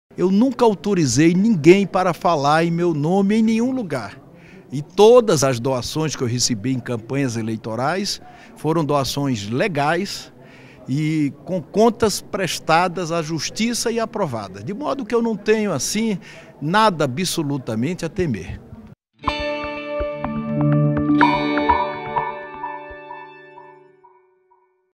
Renan nega irregularidade em campanhas eleitorais
O presidente do Senado, Renan Calheiros, reafirmou nesta quarta-feira (15) que todas as doações de campanha que recebeu são legais e estão registradas nas prestações de contas. Disse, também, que nunca autorizou alguém a falar em seu nome. As declarações ocorreram em resposta a conteúdo divulgado da delação premiada do ex-presidente da Transpetro, Sérgio Machad